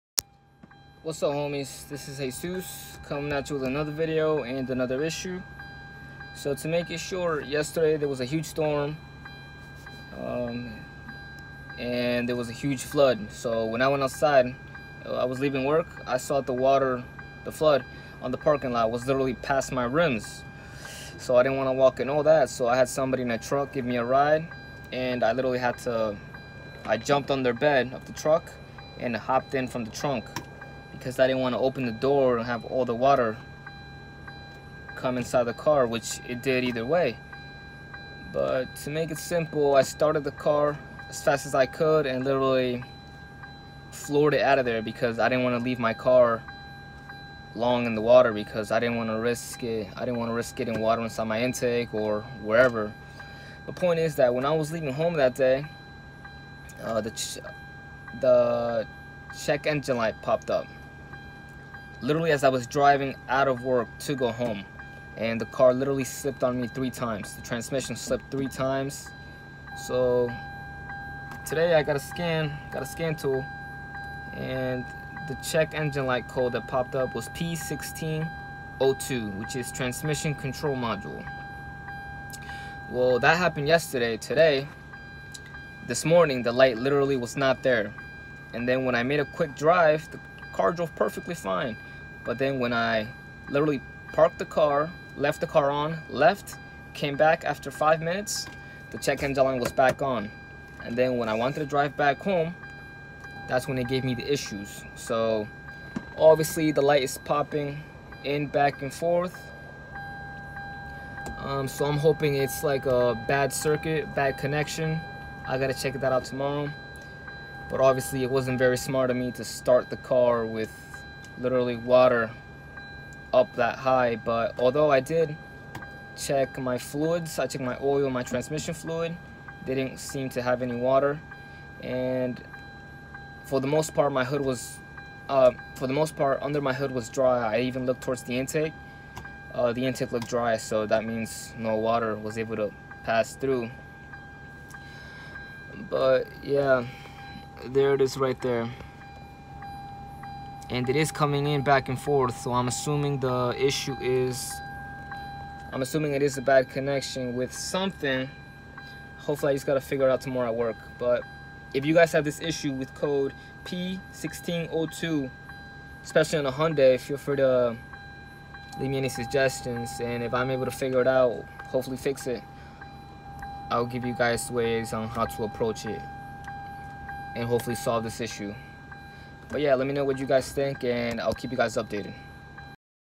Check Engine Light Code P1602
Possibly due to a flood, my check engine light popped up with code P 1602. My Hyundai has been slipping for the past two days, in other words when I hit the gas pedal, I get no speed but my RPMs get higher and higher when in drive.